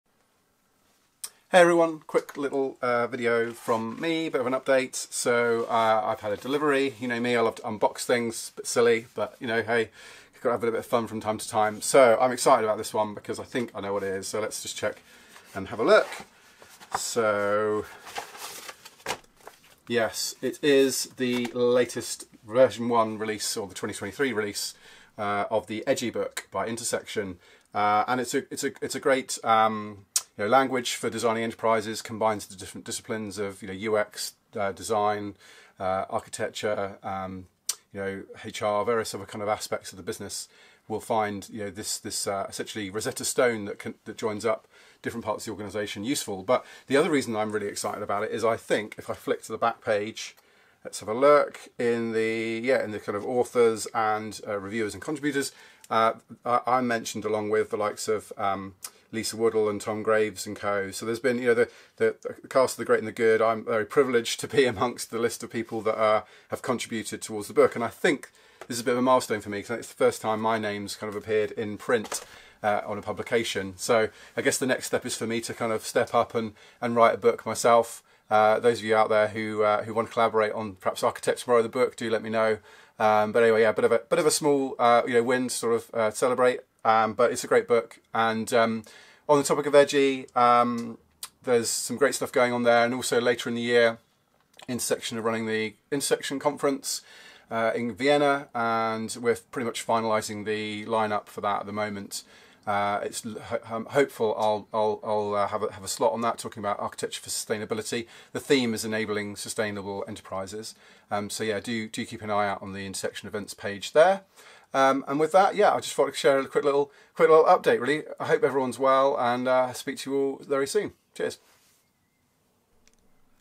Unboxing EDGY the book - a milestone for Intersection Group and Oliver
Quick update - Intersection Group's latest book EDGY is now available in print.

The book is available to order or download the PDF for free

The Intersection conference in Vienna later this year - tickets are going fast for this.